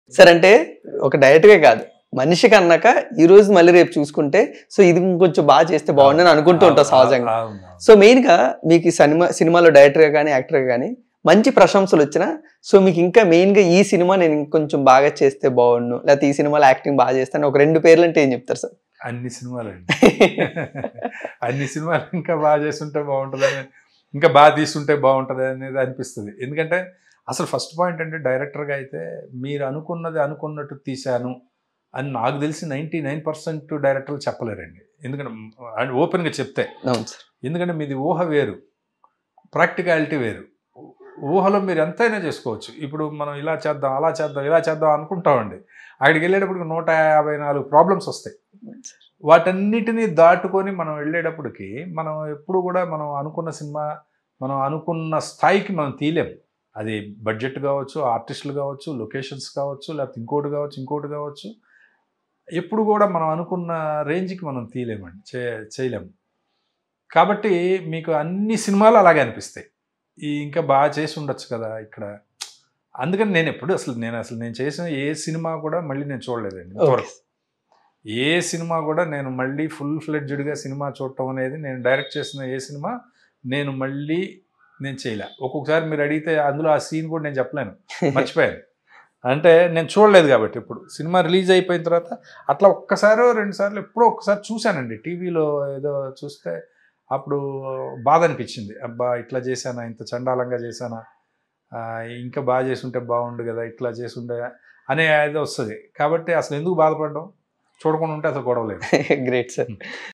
Actor & Director Devi Prasad About His Mistakes And Regret Towards His Films | Mana Stars
Watch Actor & Director DeviPrasad About His Mistakes And Regret Towards His Films